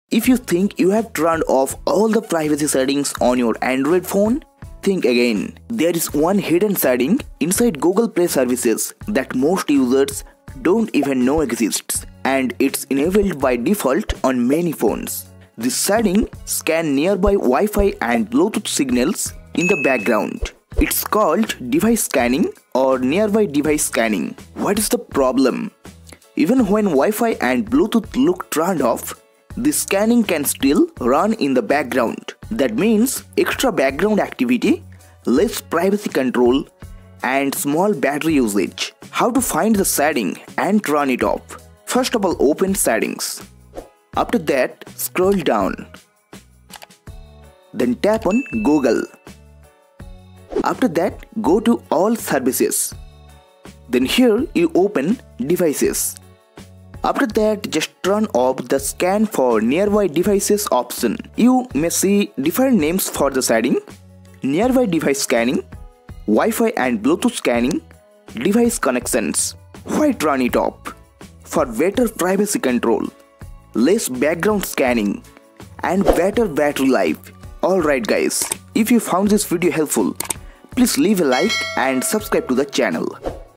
Android Users: Turn Off This Hidden Signal Scanner Now
Your Android phone has a hidden setting inside Google Play Services that most users don’t even know exists — and it’s enabled by default on many phones.
This setting quietly scans nearby Wi-Fi and Bluetooth signals, even when they appear to be turned off. While it can be useful for certain Android features, many users prefer to control it for better privacy and battery life.
In this video, I’ll show you:
• What the hidden signal scanning feature is (Device Scanning / Nearby Device Scanning)
• Why it runs in the background even when Wi-Fi and Bluetooth look off
• The potential disadvantages of leaving it ON (background scanning, reduced privacy, small battery drain)
• How to locate the setting on your phone and turn it OFF step by step
• The different names it may appear under:
  • Nearby Device Scanning
  • Wi-Fi and Bluetooth Scanning
  • Device Connections
• Why turning it OFF is beneficial (better privacy, less background activity, improved battery life).
This guide works for most Android devices including Samsung, Pixel, Motorola, Xiaomi, and others.
⚠️ Turning this OFF does not affect calls, internet, or app functionality. Some nearby features like Nearby Share may connect slightly slower.
If you found this video helpful:
💬 Comment your phone model if you need help.